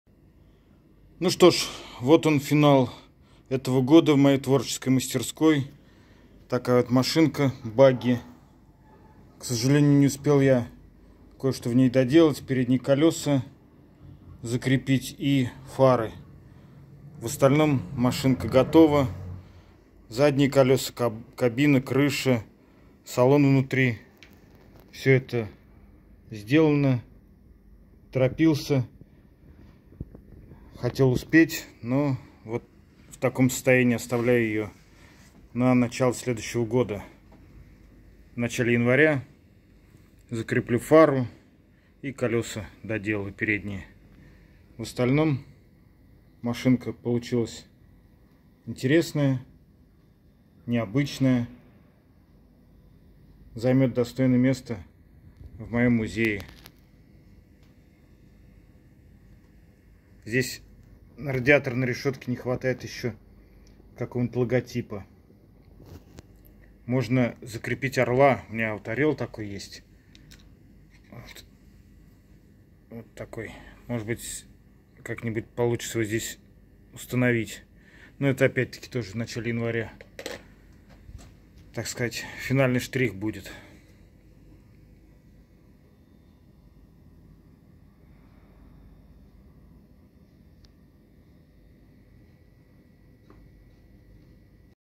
Итог года в моей творческой мастерской
Можно сказать, что очередной экспонат для моего музея готов. На нём завершаю год в своей творческой мастерской